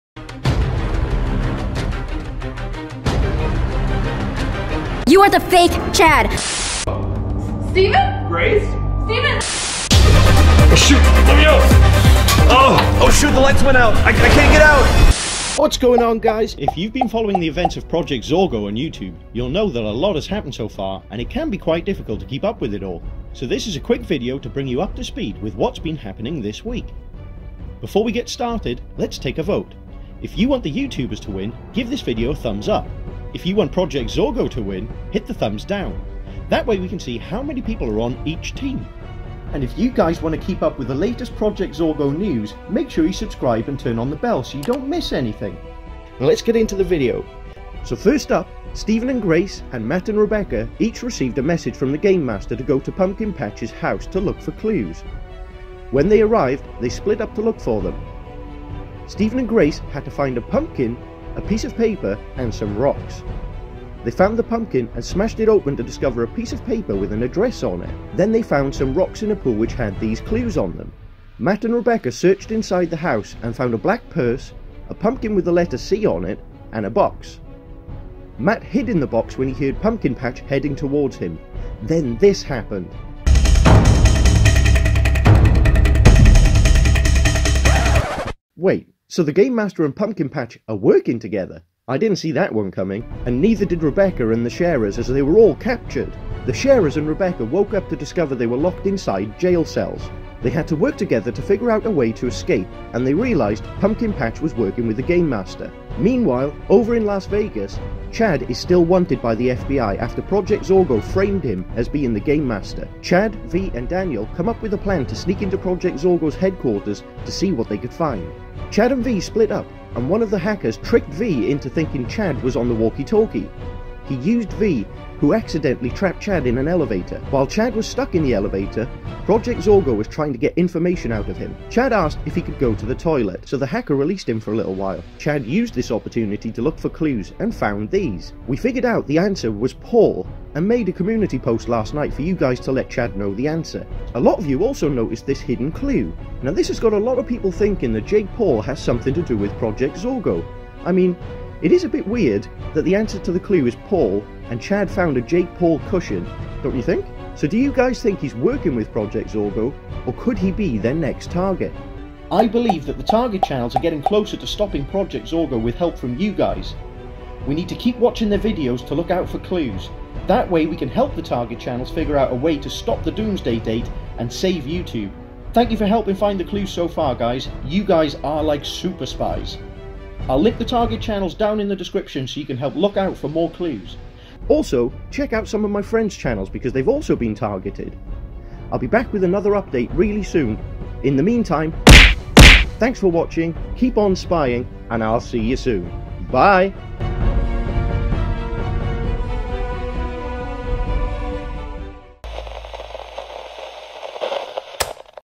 PROJECT ZORGO News - Did JAKE PAUL Trap CHAD WILD CLAY?!
In this video, we'll be giving you an update on some of the Project Zorgo events that you may have missed in the last few days and showing you clues that Jake Paul may be working for Project Zorgo!

We'll also be covering events including:

⭐Rebecca Zamolo and Matt looking forclues at Pumpkin Patch's house.

⭐Stephen and Grace Sharer try to escape jail.

⭐The Game Master and Pumpkin Patch working together!

⭐Chad Wild Clay and Vy Qwaint searching for Project Zorgo's HQ and find clues that Jake Paul may be a member of Project Zorgo!